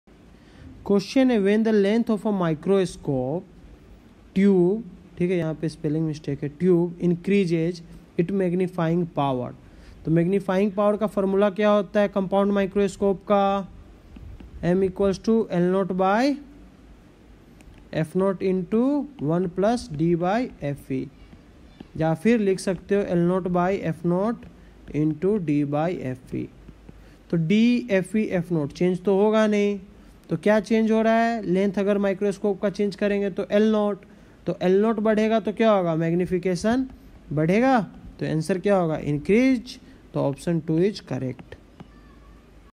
Basic Question || Magnifying Power of Compound Microscope || Ray Optics, NEET JEE
When the length of a microscope tube increases, its magnifying power
 (1) decreases (2) increases
 (3) does not change (4) can’t say